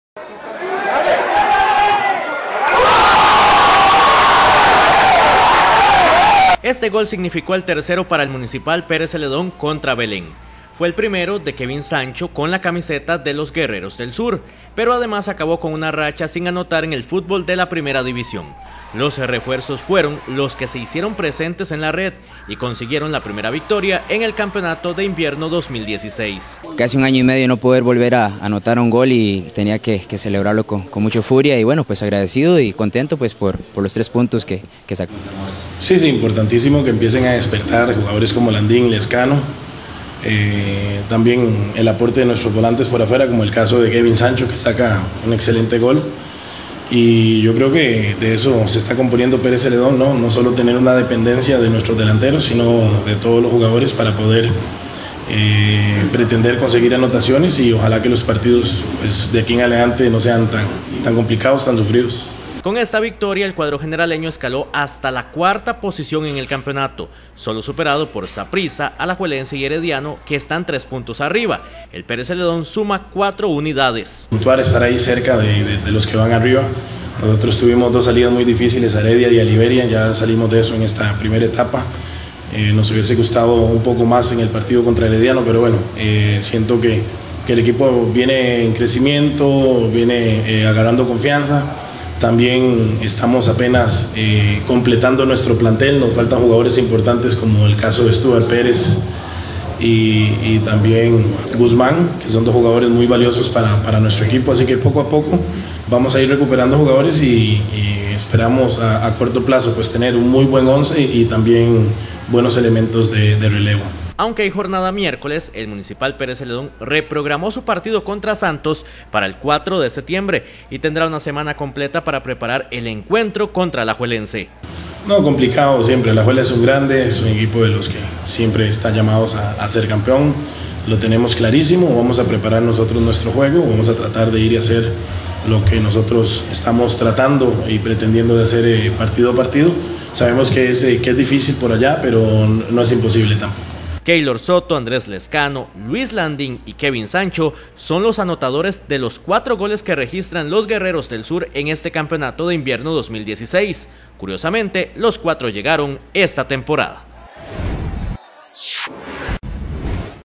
Kevin Sancho pasó año y medio para volver a celebrar un gol
El tercer gol del Municipal Pérez Zeledón contra Belén, fue el primero de Kevin Sancho con la camiseta de los guerreros del sur, pero además acabó con una racha sin anotar en el fútbol de la primera división, los refuerzos fueron los que se  hicieron presentes en la red  y consiguieron la primera victoria en el campeonato de invierno 2016.